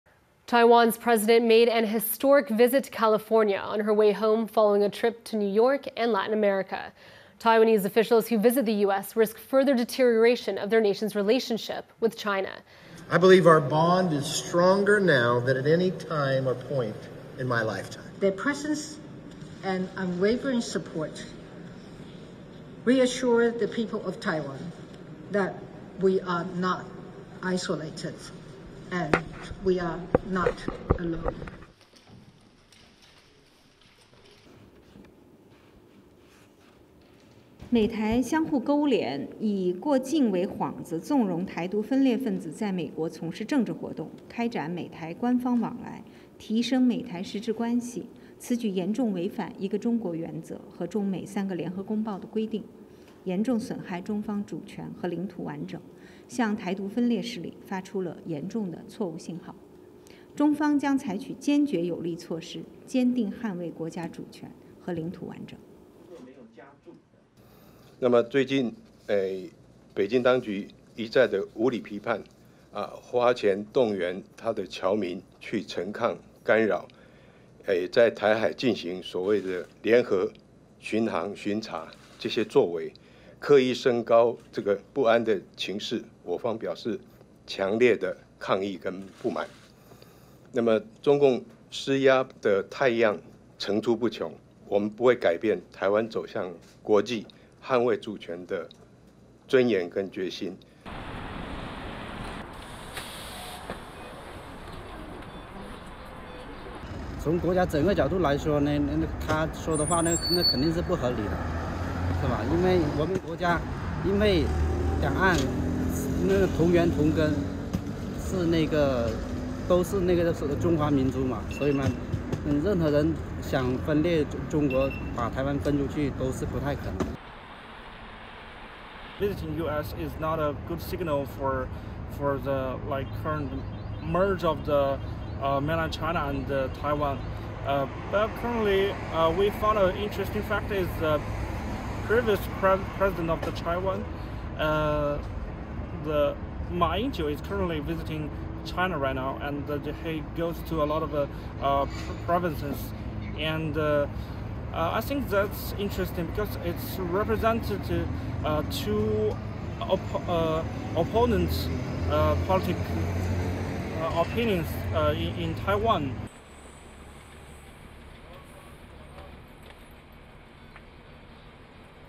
China denounces US-Taiwan meeting, says it sends "seriously wrong signal"
China's foreign ministry denounced a meeting between U.S. House Speaker Kevin McCarthy and Taiwanese President Tsai Ing-Wen, saying the two countries were "colluding with each other."

"This move seriously violates the One-China principle... and sends a seriously wrong signal to the Taiwan independence separatist forces," Chinese foreign affairs spokesperson Mao Ning said on Thursday.

"Taiwan officials are bracing for intensifying pressure on the "median line" that has for decades helped keep the peace in the Taiwan Strait, as China begins inspecting civilian shipping across the waterway, which is seen as retaliation for the meeting.

A Chinese aircraft carrier which Taiwan's defence ministry said was also spotted off Taiwan's east coast.